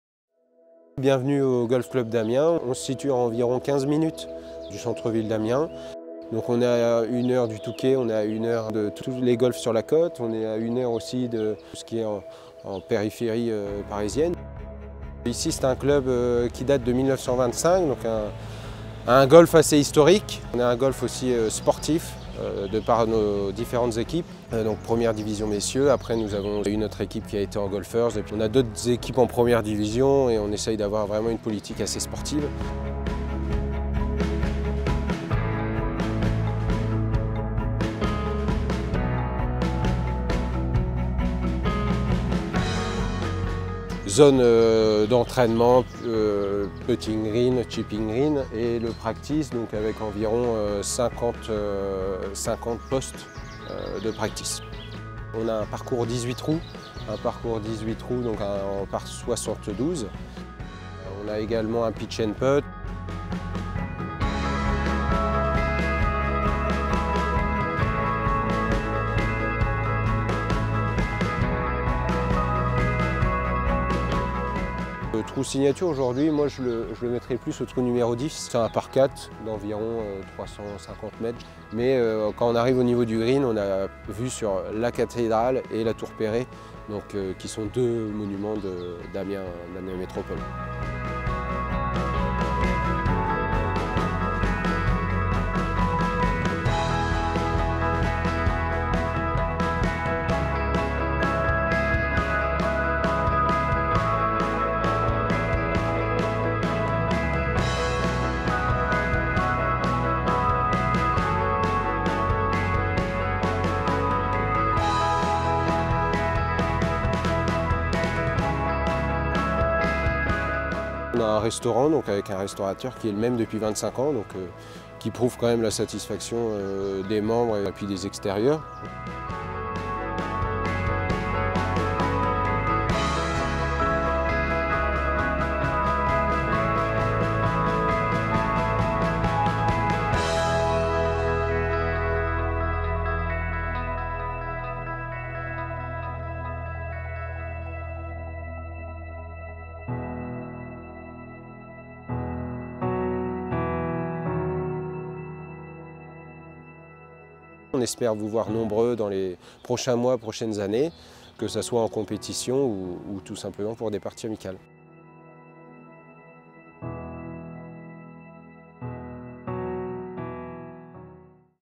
Golf Club D'amiens - drone aerial video - Overview
A quelques minutes du centre d'Amiens, le golf club d’Amiens constitue un symbole parfait de l'art de vivre en Picardie. Son parcours se déroule agréablement en bordure du bois de Querrieu. Si les premiers trous sont apparus dès 1925, le parcours connaît sa configuration actuelle depuis 1977.
Ce dernier, plat et boisé, est technique. Effectuez vos mises en jeu avec réflexion, les attaques de green seront plus aisées. Le sous-sol crayeux assure un bon drainage permettant le jeu au sec toute l’année.